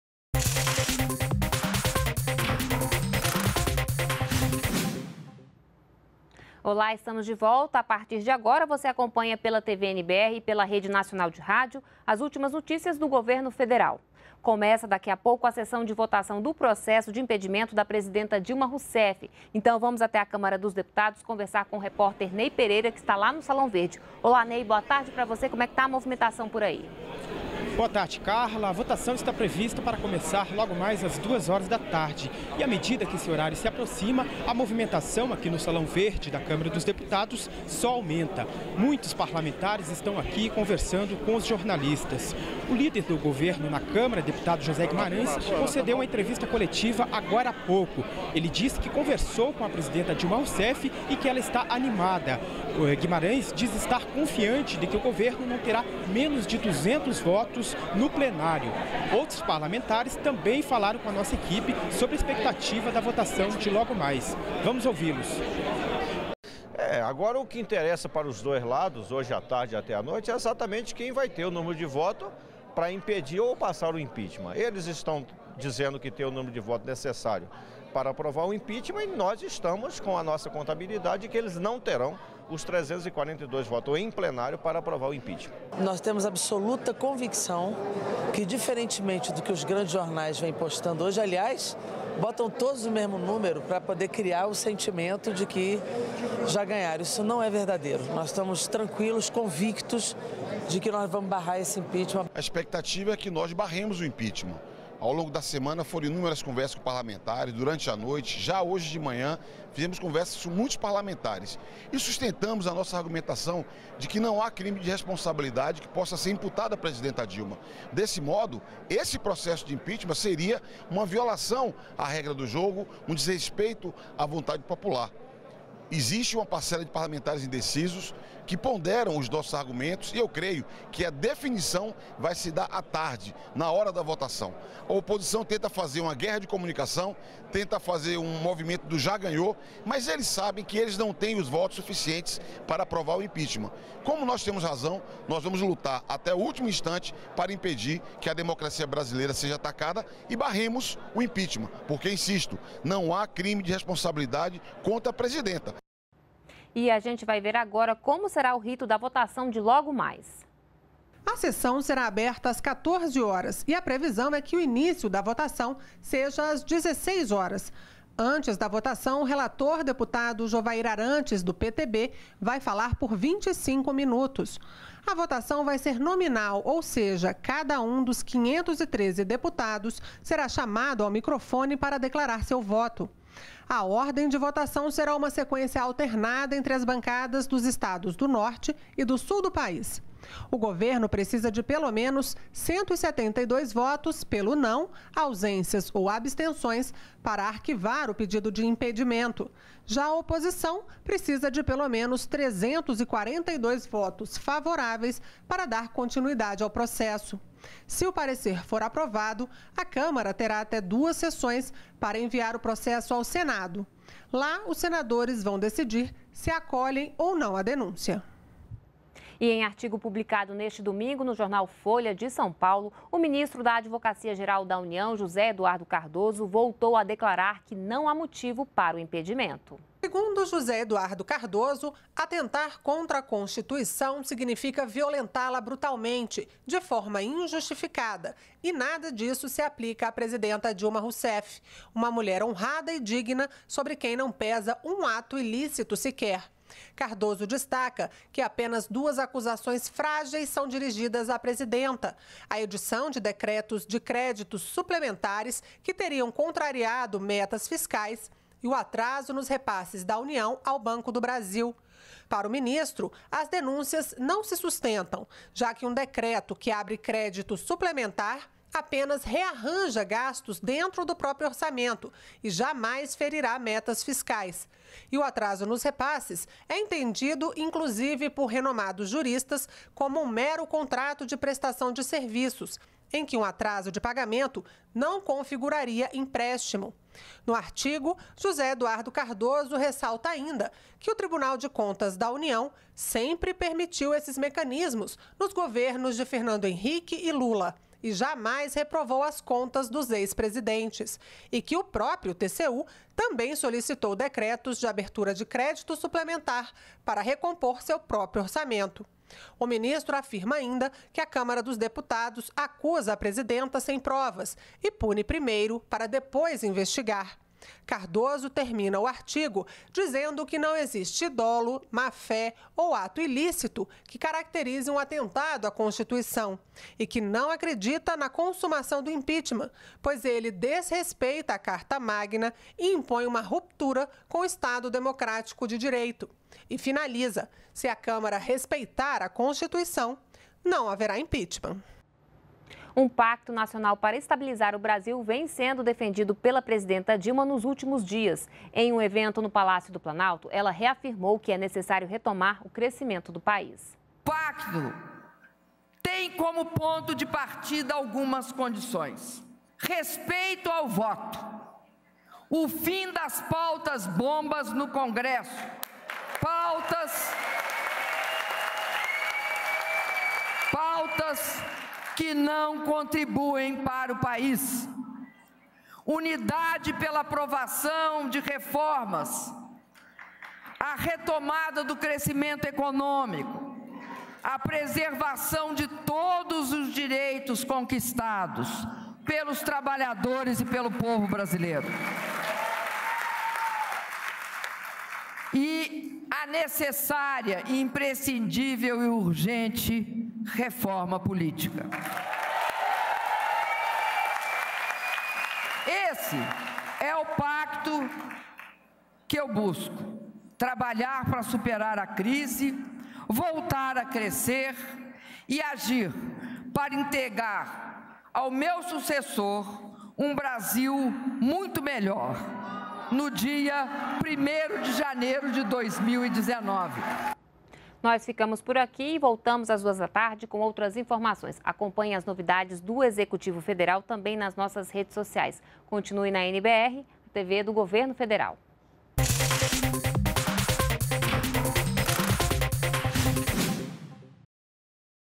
Últimas - Parlamentares votam hoje processo de impeachment contra a presidenta Dilma Rousseff
ÚLTIMAS 13H - 17.04.16: Nesta tarde, a Câmara dos Deputados vota o processo de impeachment contra a presidenta Dilma Rousseff. Em entrevista à TV NBR, o parlamentar Orlando Silva (PCdoB - SP) reafirmou que não há crime de responsabilidade contra a presidenta e disse que a aprovação do processo será um desrespeito à vontade popular. Para o processo ser aprovado, é necessário que 342 deputados aprovem o parecer do relator Jovair Arantes (PTB-GO).